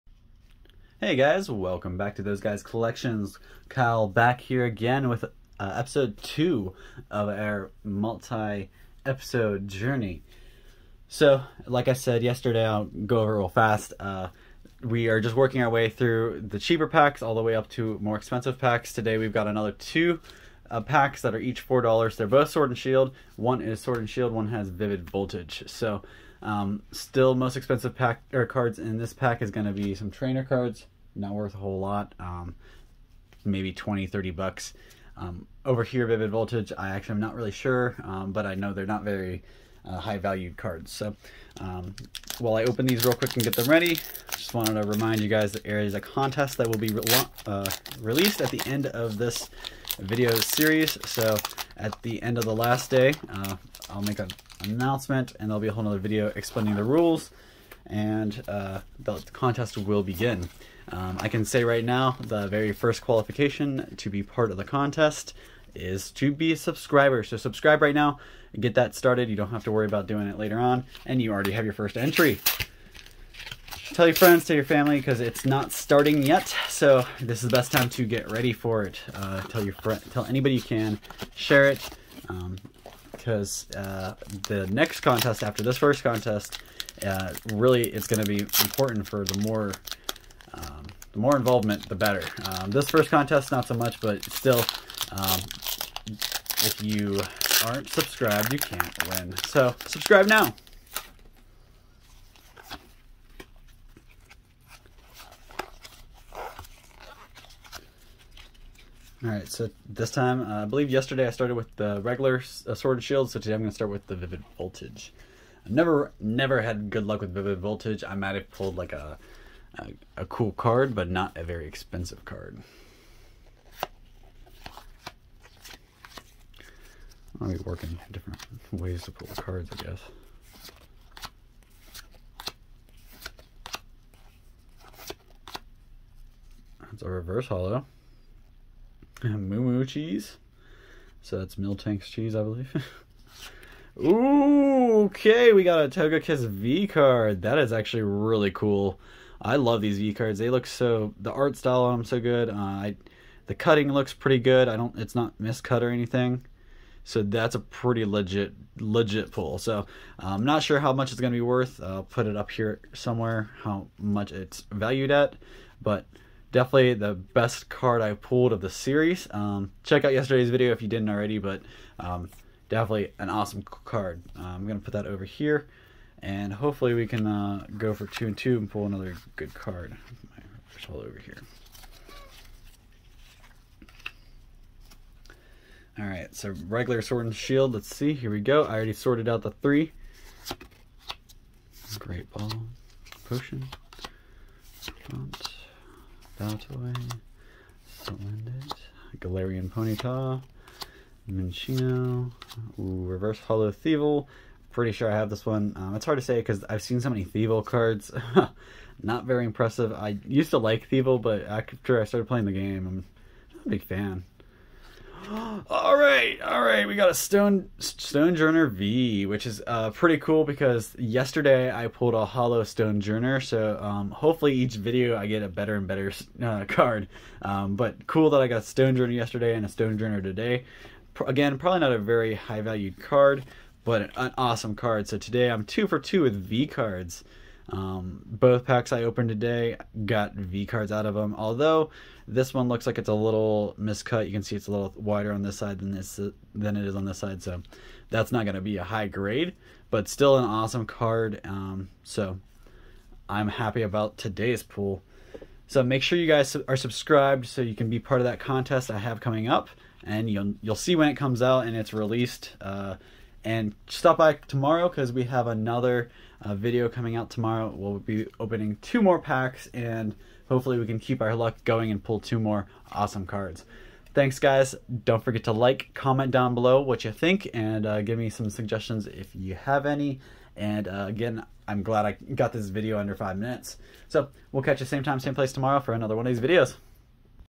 Journey Through Assorted Pokemon Packs! Episode 2 ~ Sword & Shield & Vivid Voltage Packs!
Follow the link below for Episode 3!